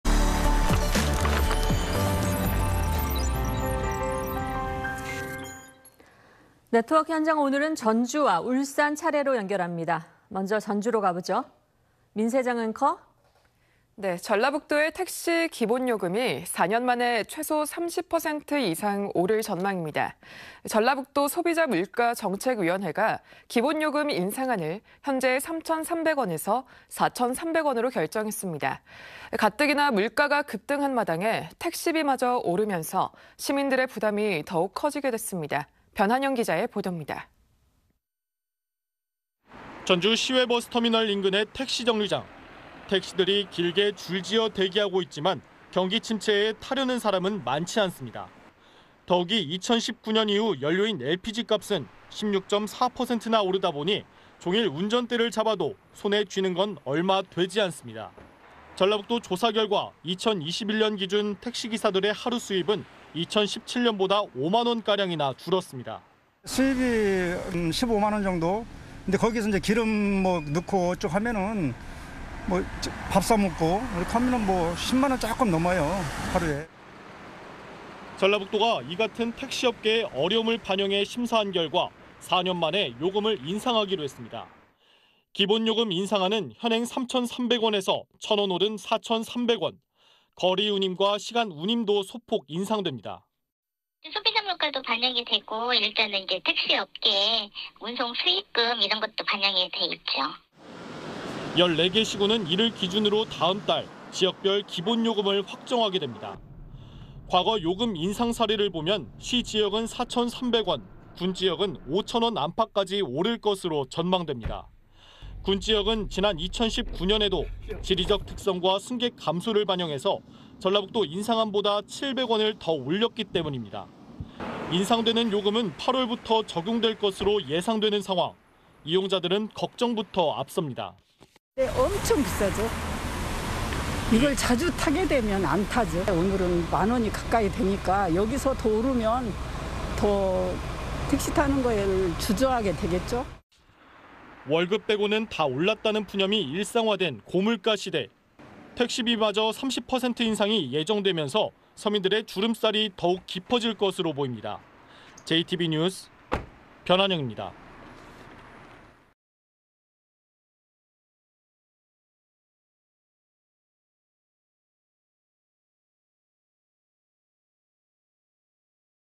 전북도, 4년 만에 택시 요금 인상…3,300원→4,300원 / SBS
〈앵커〉

전라북도의 택시 기본요금이 4년 만에 최소 30% 이상 오를 전망입니다. 전라북도 소비자물가 정책위원회가 기본요금 인상안을 현재의 3천300원에서 4천300원으로 결정했습니다. 가뜩이나 물가가 급등한 마당에 택시비마저 오르면서 시민들의 부담이 더욱 커지게 됐습니다.

변한영 기자의 보도입니다.

〈기자〉

전주 시외버스터미널 인근의 택시 정류장.

택시들이 길게 줄지어 대기하고 있지만 경기침체에 타려는 사람은 많지 않습니다.

더욱이 2019년 이후 연료인 LPG 값은 16.4%나 오르다 보니 종일 운전대를 잡아도 손에 쥐는 건 얼마 되지 않습니다.

전라북도 조사 결과 2021년 기준 택시 기사들의 하루 수입은 2017년보다 5만 원가량이나 줄었습니다.

[최화석/택시 기사 : 수입이 (15시간 운전하면) 한 15만 원 정도. 근데 거기에서 이제 기름 넣고, 밥 사 먹고 이렇게 하면 10만 원 조금 넘어요, 하루에.]

전라북도가 이 같은 택시업계의 어려움을 반영해 심사한 결과 4년 만에 요금을 인상하기로 했습니다.

기본요금 인상안은 현행 3천300원에서 1천 원 오른 4천300원.

거리 운임과 시간 운임도 소폭 인상됩니다.

[전라북도 관계자 : 소비자 물가도 반영이 되고 일단은 이제 택시업계 운송 수익금 이런 것도 반영돼 있죠.]

14개 시군은 이를 기준으로 다음 달 지역별 기본요금을 확정하게 됩니다.

과거 요금 인상 사례를 보면 시 지역은 4천300원, 군 지역은 5천원 안팎까지 오를 것으로 전망됩니다.

군지역은 지난 2019년에도 지리적 특성과 승객 감소를 반영해서 전라북도 인상안보다 700원을 더 올렸기 때문입니다.

인상되는 요금은 8월부터 적용될 것으로 예상되는 상황.

이용자들은 걱정부터 앞섭니다.

[김찬숙/택시 이용객 : 엄청 비싸죠. 이걸 자주 타게 되면 안 타죠. 오늘 (택시비가) 1만 원 가까이 되니까 여기서 더 오르면 더 택시 타는 걸 주저하게 되겠죠.]

월급 빼고는 다 올랐다는 푸념이 일상화된 고물가 시대.

택시비마저 30% 인상이 예정되면서 서민들의 주름살이 더욱 깊어질 것으로 보입니다.

(영상취재 : 이동녕 JTV)

JTV 변한영